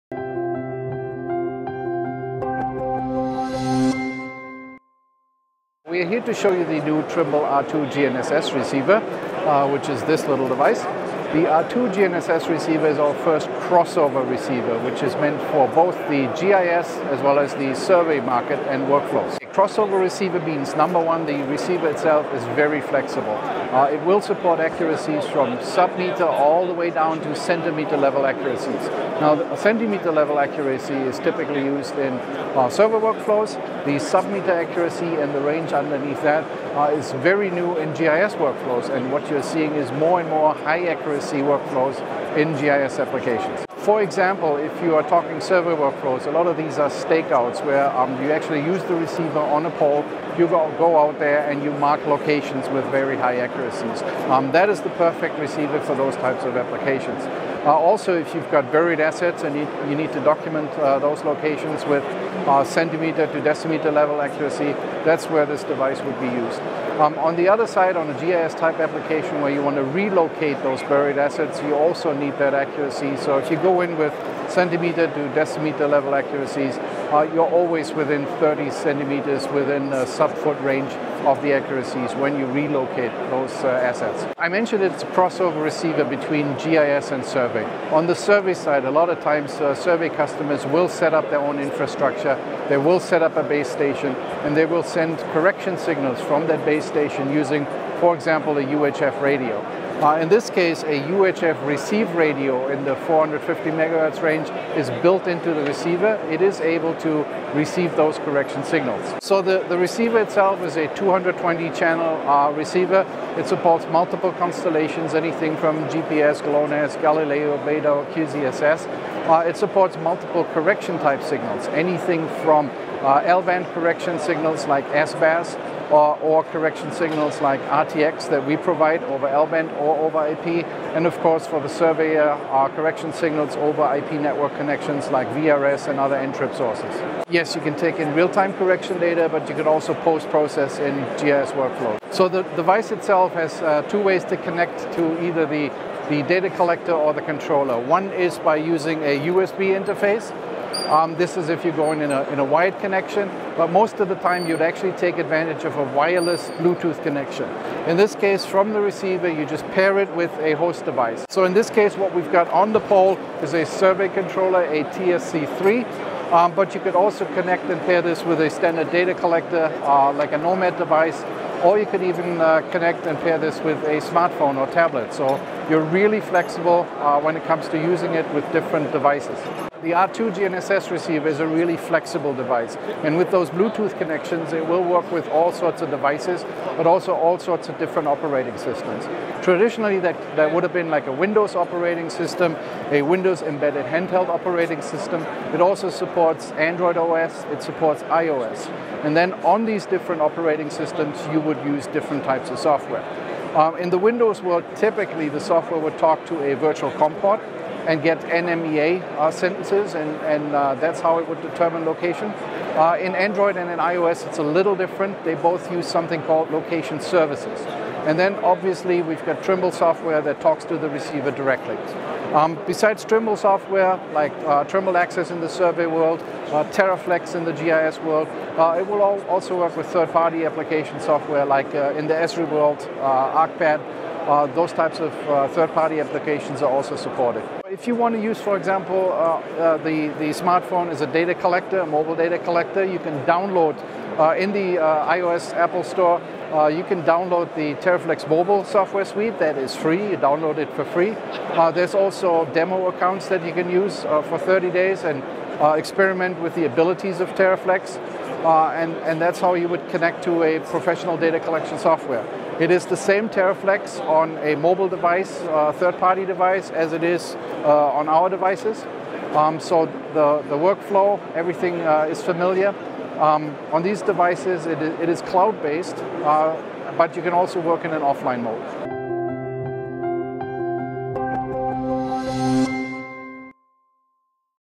All new Trimble R2 GNSS receiver
Matthias H.  Van Doorn, Product Manager, Geospatial Division, Trimble talks about the new product for surveying and mapping